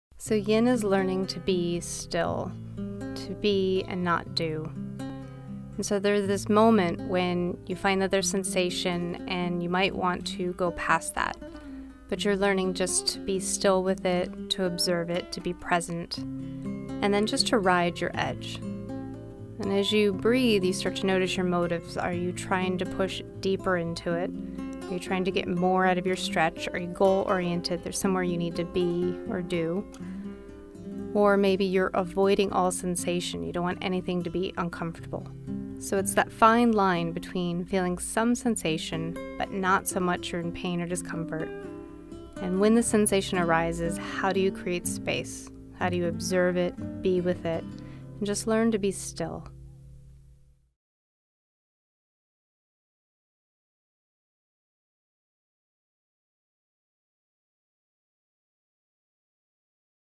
Learning to Be and Not Do
There is great reward in stillness. Yin yoga helps us find and observe the small sensations in order to practice presence.

Online yoga classes with Jennifer Elliott are now available on Wanderlust TV